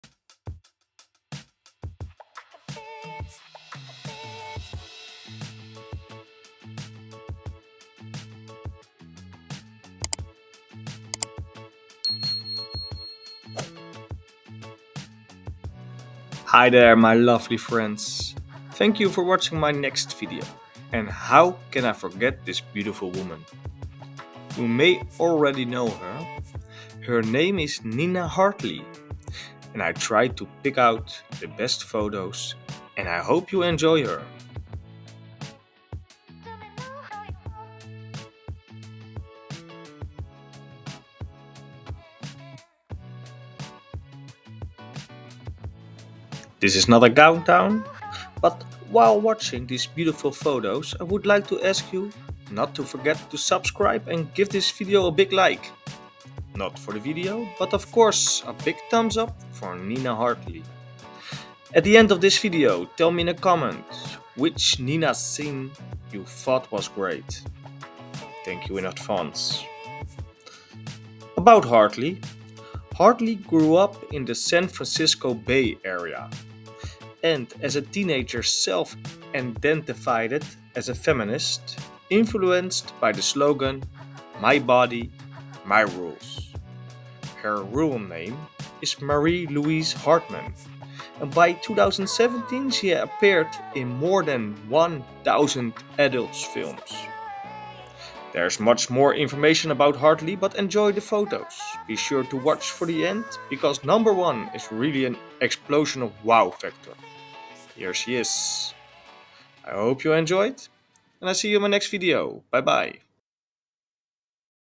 MUST SEE: The Beautiful NINA HARTLEY OVER 50 | Amazing Woman
Who doesn't know her? This Beautiful Woman has starred in thousands of movies. Of course, we are talking about this Amazing and Lovely woman, Nina Hartley. This OVER 50 Woman has much more information. Therefore be sure to watch my video till the end. I have selected the 10 Best Photos for your eyes only.